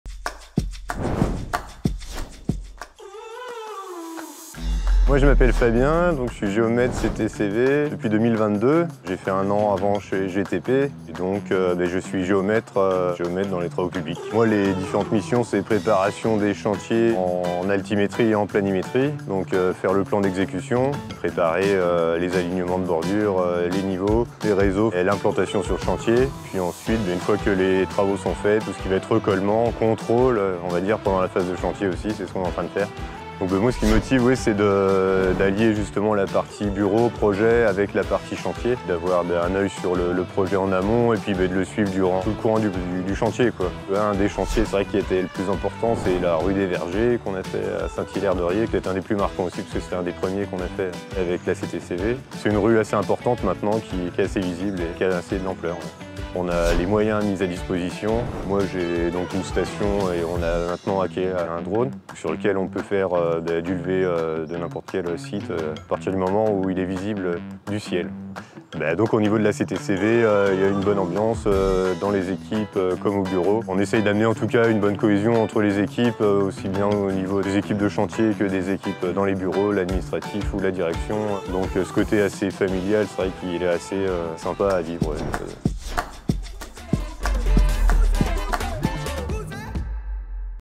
Portrait collaborateur / Fabien - Géomètre chez CTCV TP
Découvrez notre série "portrait collaborateur" avec Fabien, géomètre chez CTCV TP témoignant sur son poste.